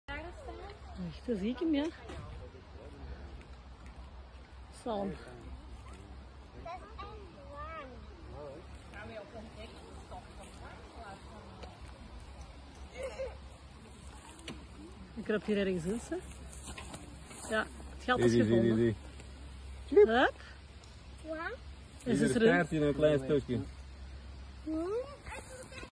Ringslang
Ringslang – Natrix natrix – Grass snake
Ringslangen danken hun naam aan 2 lichte, halvemaanvormige vlekken achter de kop, die in de nek aansluiten zodat ze een soort "ring" vormen om de hals. De Ringslang is het grootste reptiel in ons land en vrij zeldzaam. Mannelijke Ringslangen kunnen 90 cm worden, vrouwelijke 120 cm. Ze eten kleine insecten, vissen en amfibieën. Ze zijn ongevaarlijk en niet giftig voor de mens.
Ringslangen worden aangetroffen in de nabijheid van water. Ze foerageren in moerasgebieden, natte graslanden en broekbossen. Bij deze plaatsen moeten ook voldoende schuilplaatsen zijn en plaatsen om te zonnen. Hun eieren leggen Ringslangen in zogenaamde broedhopen. Dit zijn hopen rottend plantaardig materiaal waarin de eieren worden uitgebroed door de warmte die bij de rotting vrijkomt.
Ringslang gefilmd door Leo PEETERS op het Kempisch kanaal.